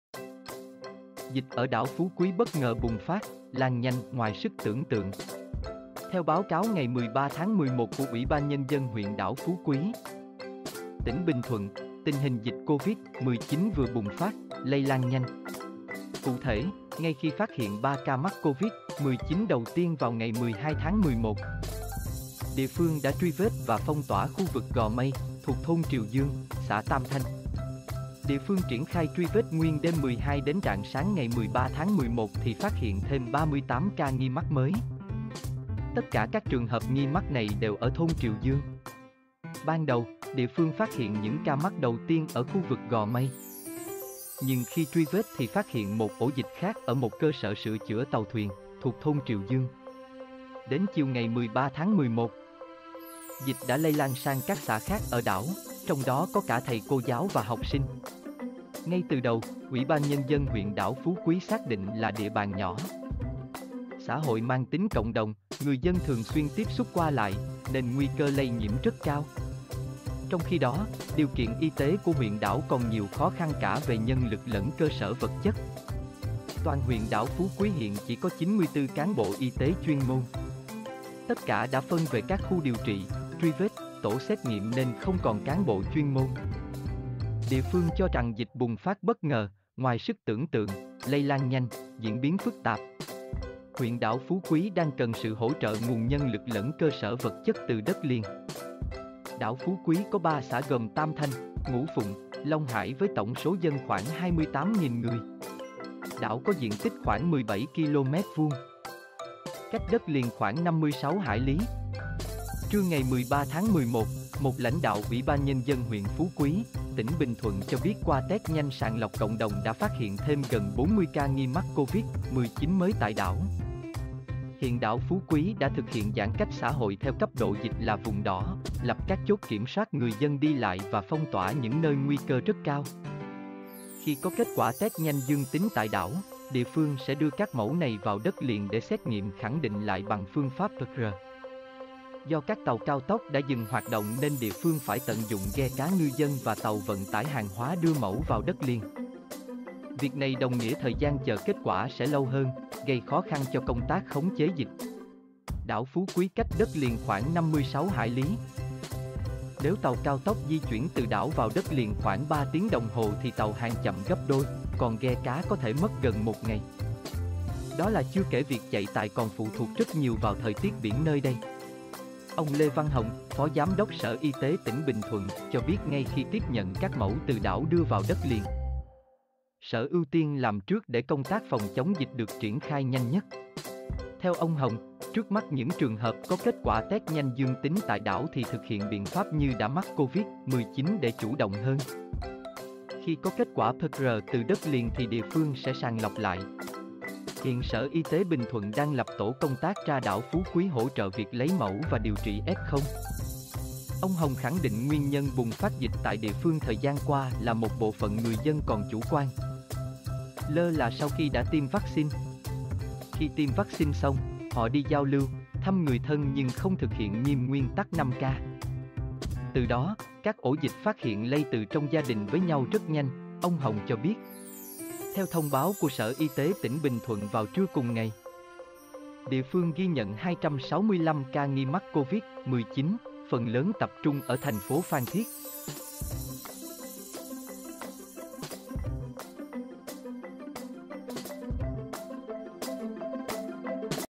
Dịch ở đảo Phú Quý bất ngờ bùng phát, lan nhanh 'ngoài sức tưởng tượng'
Theo báo cáo ngày 13-11 của UBND huyện đảo Phú Quý, tỉnh Bình Thuận, tình hình dịch COVID-19 vừa bùng phát, lây lan nhanh.

Cụ thể, ngay khi phát hiện 3 ca mắc COVID-19 đầu tiên vào ngày 12-11, địa phương đã truy vết và phong tỏa khu vực gò Mây, thuộc thôn Triều Dương, xã Tam Thanh.

Địa phương triển khai truy vết nguyên đêm 12 đến rạng sáng 13-11 thì phát hiện thêm 38 ca nghi mắc mới. Tất cả các trường hợp nghi mắc này đều ở thôn Triều Dương.

Ban đầu, địa phương phát hiện những ca mắc đầu tiên ở khu vực gò Mây. Nhưng khi truy vết thì phát hiện một ổ dịch khác ở một cơ sở sửa chữa tàu thuyền, thuộc thôn Triều Dương.

Đến chiều 13-11, dịch đã lây lan sang các xã khác ở đảo, trong đó có cả thầy cô giáo và học sinh.

Ngay từ đầu, UBND huyện đảo Phú Quý xác định là địa bàn nhỏ, xã hội mang tính cộng đồng (người dân thường xuyên tiếp xúc qua lại) nên nguy cơ lây nhiễm rất cao.

Trong khi đó, điều kiện y tế của huyện đảo còn nhiều khó khăn cả về nhân lực lẫn cơ sở vật chất.

Toàn huyện đảo Phú Quý hiện chỉ có 94 cán bộ y tế chuyên môn. Tất cả đã phân về các khu điều trị, truy vết, tổ xét nghiệm nên không còn cán bộ chuyên môn.

Địa phương cho rằng dịch bùng phát bất ngờ, ngoài sức tưởng tượng, lây lan nhanh, diễn biến phức tạp.

Huyện đảo Phú Quý đang cần sự hỗ trợ nguồn nhân lực lẫn cơ sở vật chất từ đất liền.

Đảo Phú Quý có 3 xã gồm Tam Thanh, Ngũ Phụng, Long Hải với tổng số dân khoảng 28.000 người. Đảo có diện tích khoảng 17km2, cách đất liền khoảng 56 hải lý.

Trưa 13-11, một lãnh đạo UBND huyện Phú Quý, tỉnh Bình Thuận cho biết qua test nhanh sàng lọc cộng đồng đã phát hiện thêm gần 40 ca nghi mắc COVID-19 mới tại đảo.

Hiện đảo Phú Quý đã thực hiện giãn cách xã hội theo cấp độ dịch là vùng đỏ, lập các chốt kiểm soát người dân đi lại và phong tỏa những nơi nguy cơ rất cao.

Khi có kết quả test nhanh dương tính tại đảo, địa phương sẽ đưa các mẫu này vào đất liền để xét nghiệm khẳng định lại bằng phương pháp PCR.

Do các tàu cao tốc đã dừng hoạt động nên địa phương phải tận dụng ghe cá ngư dân và tàu vận tải hàng hóa đưa mẫu vào đất liền. Việc này đồng nghĩa thời gian chờ kết quả sẽ lâu hơn, gây khó khăn cho công tác khống chế dịch.

Đảo Phú Quý cách đất liền khoảng 56 hải lý. Nếu tàu cao tốc di chuyển từ đảo vào đất liền khoảng 3 tiếng đồng hồ thì tàu hàng chậm gấp đôi, còn ghe cá có thể mất gần một ngày. Đó là chưa kể việc chạy tài còn phụ thuộc rất nhiều vào thời tiết biển nơi đây.

Ông Lê Văn Hồng - phó giám đốc Sở Y tế tỉnh Bình Thuận - cho biết ngay khi tiếp nhận các mẫu từ đảo đưa vào đất liền, sở ưu tiên làm trước để công tác phòng chống dịch được triển khai nhanh nhất.

Theo ông Hồng, trước mắt những trường hợp có kết quả test nhanh dương tính tại đảo thì thực hiện biện pháp như đã mắc COVID-19 để chủ động hơn. Khi có kết quả PCR từ đất liền thì địa phương sẽ sàng lọc lại.

Hiện Sở Y tế Bình Thuận đang lập tổ công tác ra đảo Phú Quý hỗ trợ việc lấy mẫu và điều trị F0.

Ông Hồng khẳng định nguyên nhân bùng phát dịch tại địa phương thời gian qua là một bộ phận người dân còn chủ quan, lơ là sau khi đã tiêm vắc xin.

"Khi tiêm vắc xin xong, họ đi giao lưu, thăm người thân nhưng không thực hiện nghiêm nguyên tắc 5K. Từ đó, các ổ dịch phát hiện lây từ trong gia đình với nhau rất nhanh", ông Hồng cho biết.

Theo thông báo của Sở Y tế tỉnh Bình Thuận vào trưa cùng ngày, địa phương ghi nhận 265 ca nghi mắc COVID-19, phần lớn tập trung ở TP Phan Thiết.